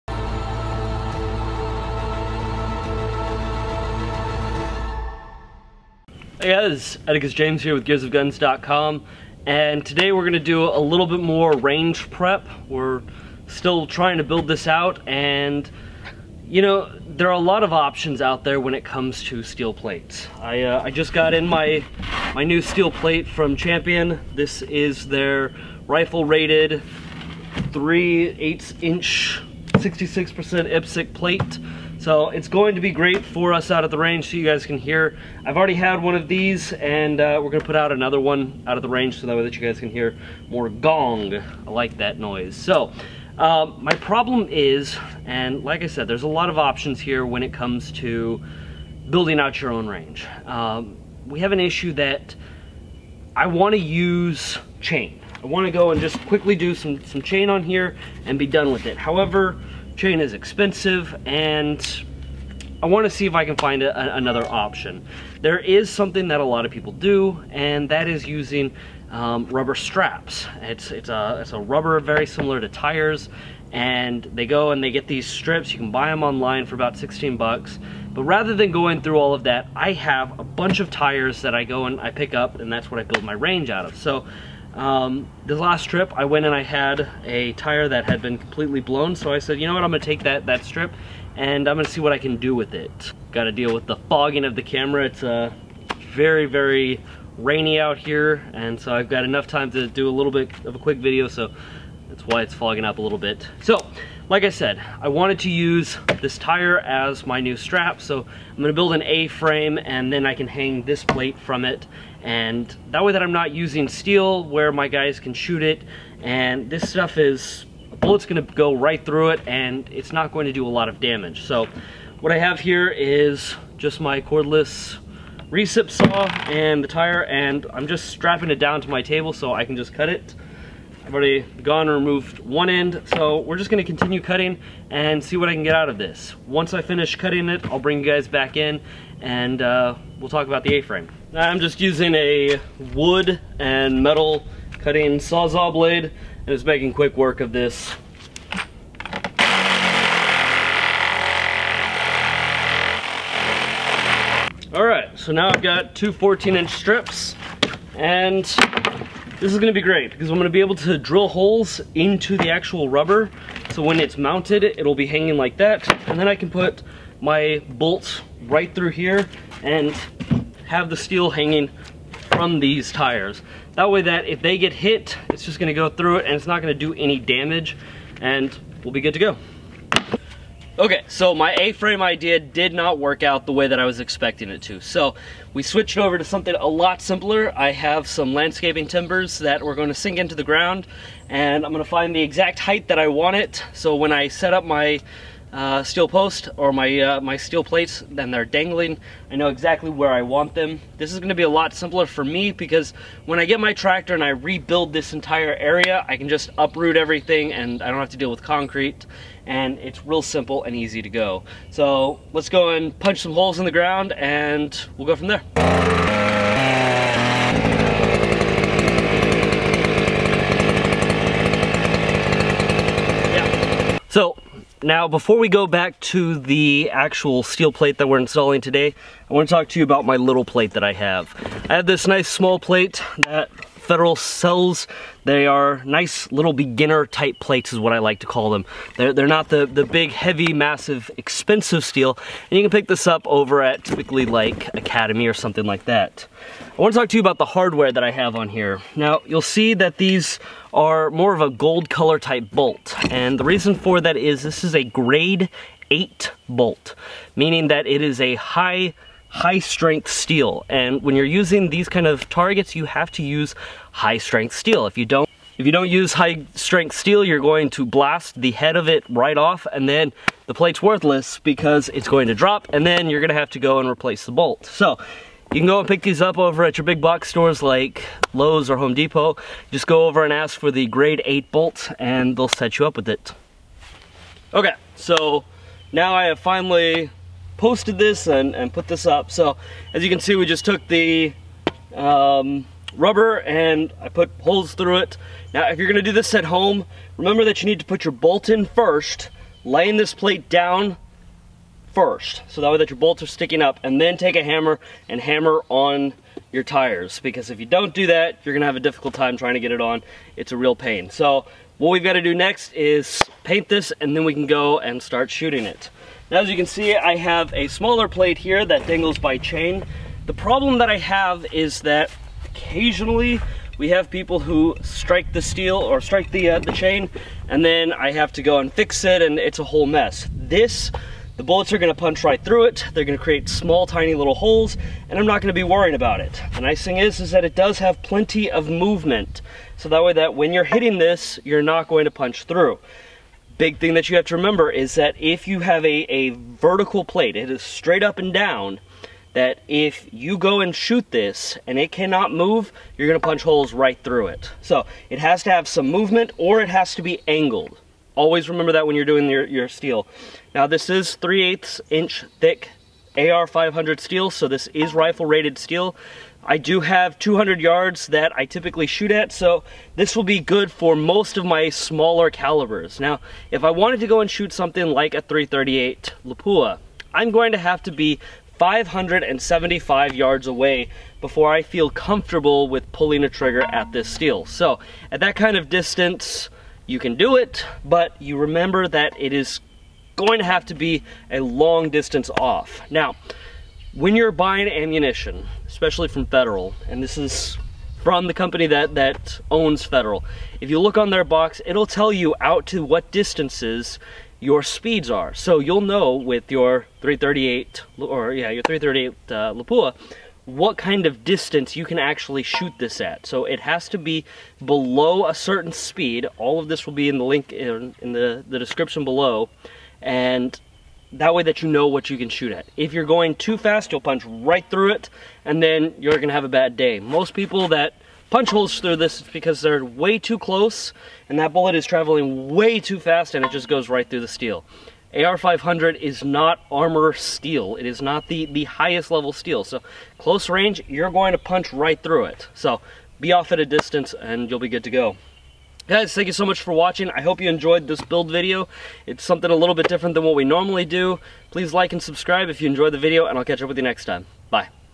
Range Build PT. 2 - Adding A Champion 66% IPSC AR500 Plate
Today I am reinstalling my Champion AR500 steel plate at the GEARS range.

As you can see this backstop isn't the biggest and it really only has room for the two plates and one paper target stand. In the coming months we will be expanding the backstop to include a pistol plate bay as well as extend the overall length to include more rifle rated steel plates.

The link below is about the steel plate and the warnings.